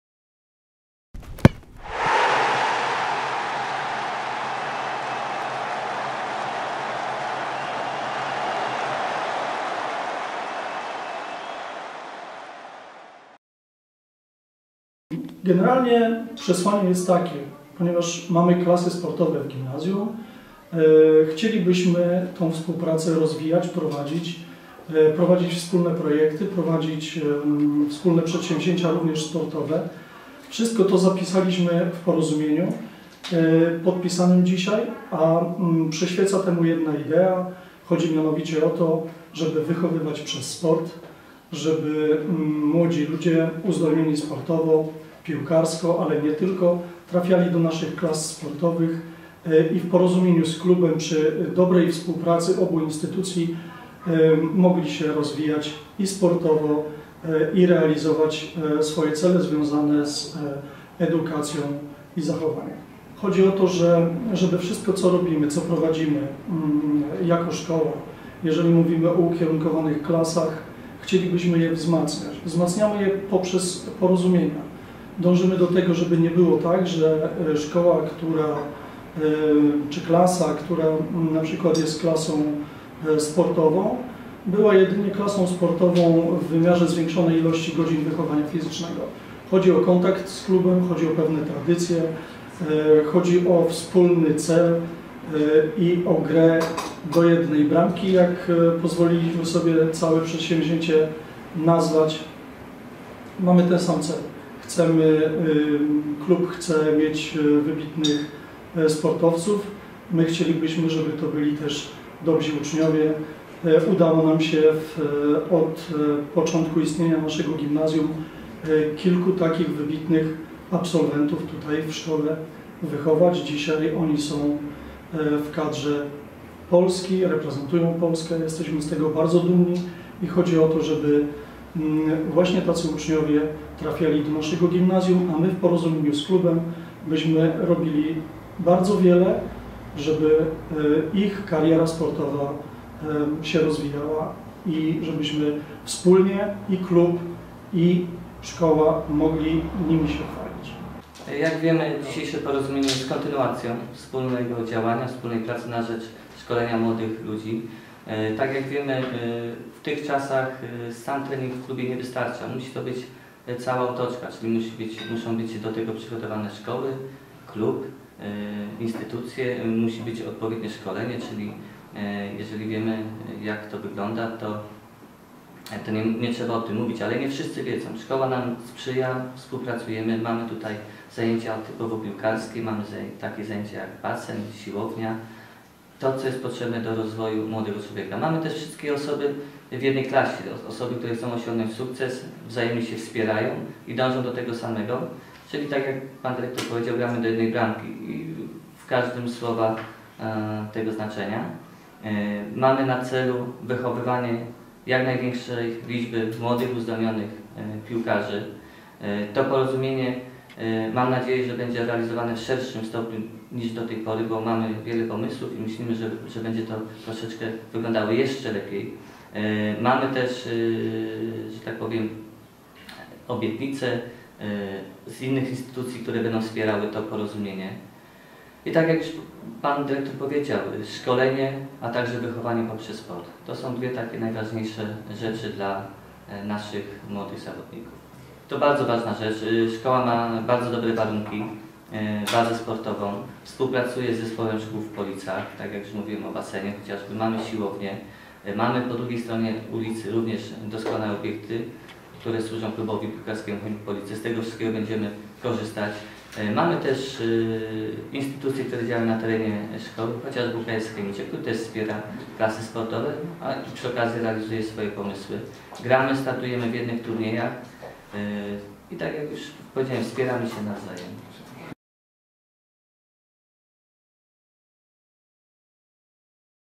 Porozumienie Chemik Police z Gimnazjum nr 3
Wypowiedzi prezesa Chemika Police i Dyrektora Gimnazjum nr 3 o podpisanym porozumieniu.